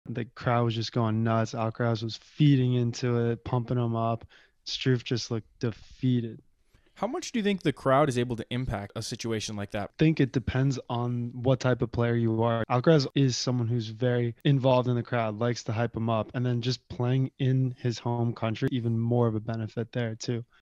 Crowd Impact in Tennis
Aidan and Erik discuss the impact of the crowd on a tennis match.

(We do not own or claim ownership to any copyrighted content in this video)